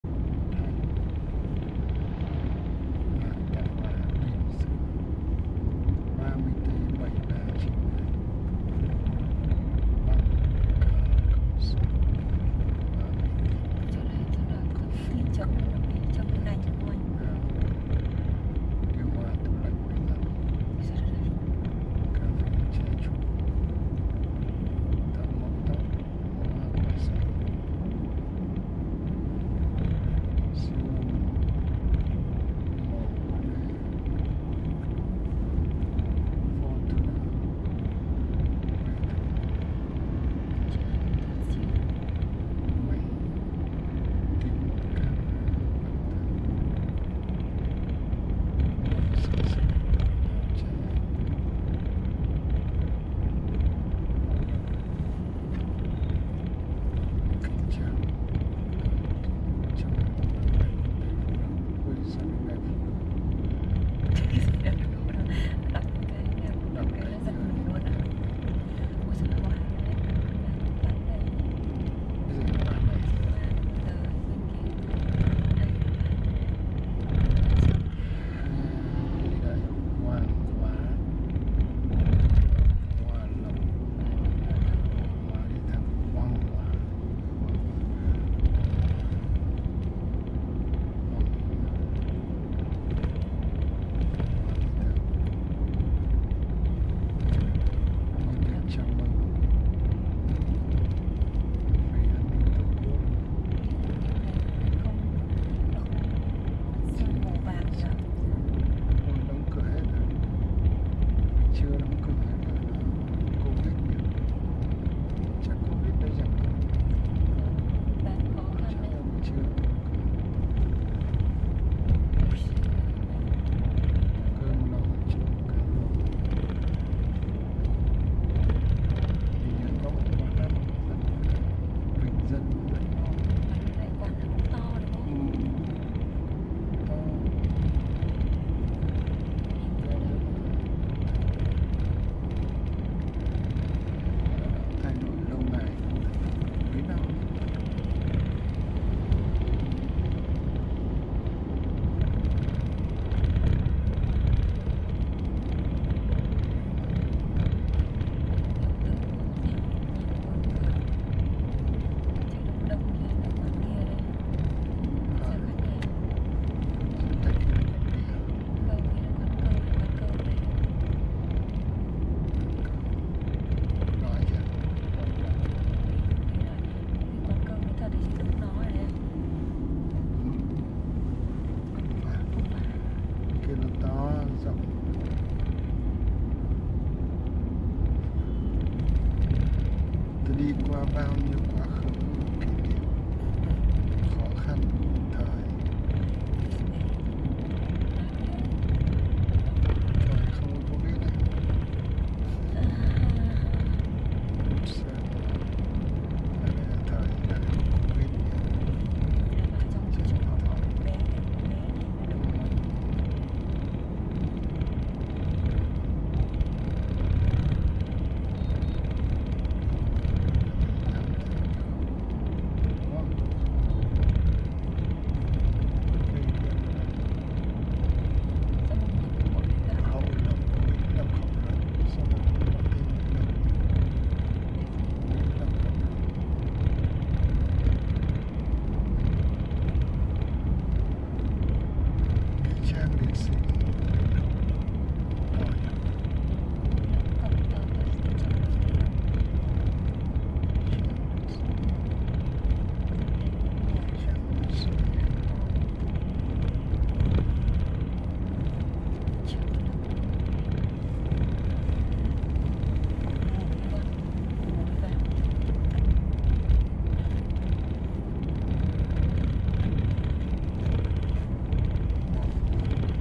Đường ở các huyện ở Thanh Hoá
NGA SƠN - HẬU LỘC - HOẰNG HOÁ (HẢI TIẾN) ĐẦU XUÂN TÂN SỬU, NĂM 2021
CAMERA NXHYT/6 MAR. 2021